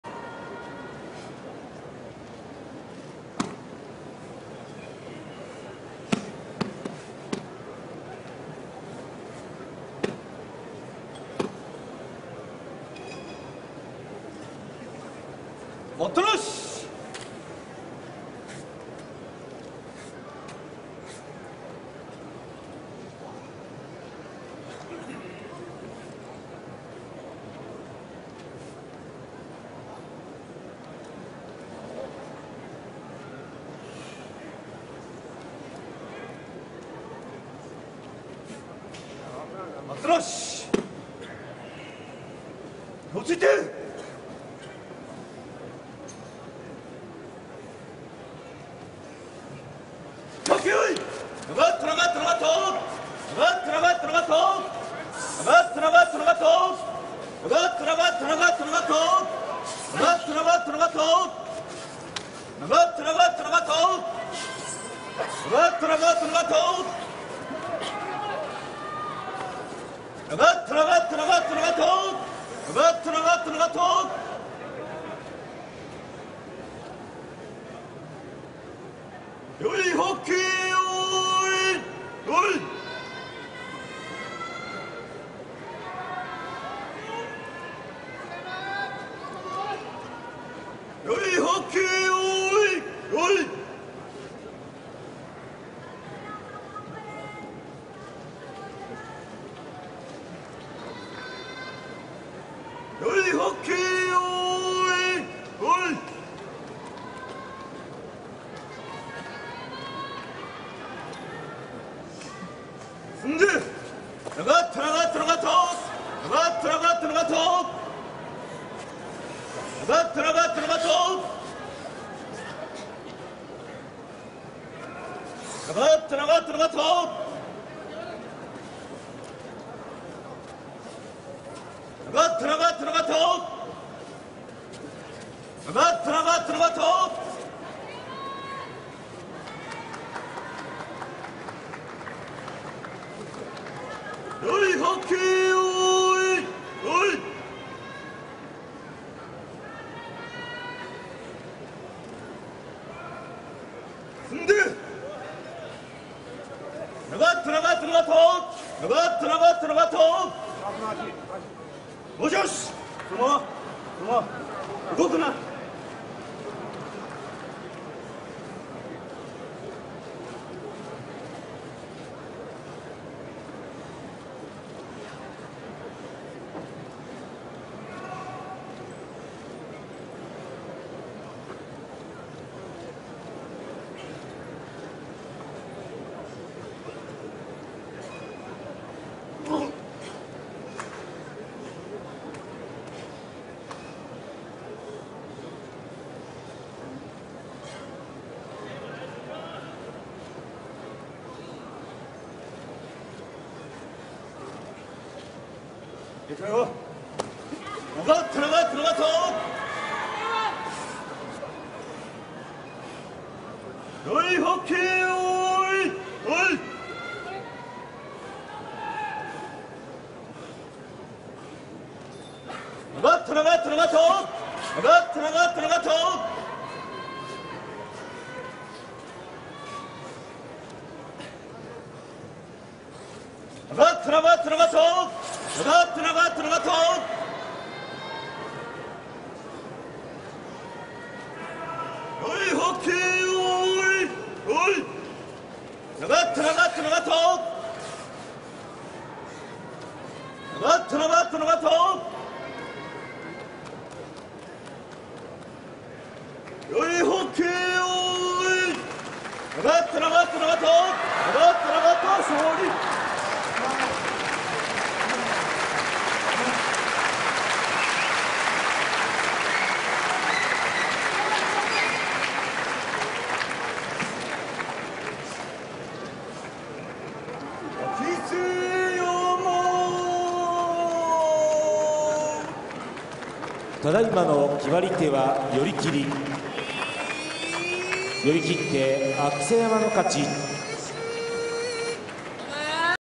里山vs明瀬山 十両取組 2013/09/24
平成２５年秋場所１０日目
廻し待ったを挟む３分近い相撲になりました。

sumo 大相撲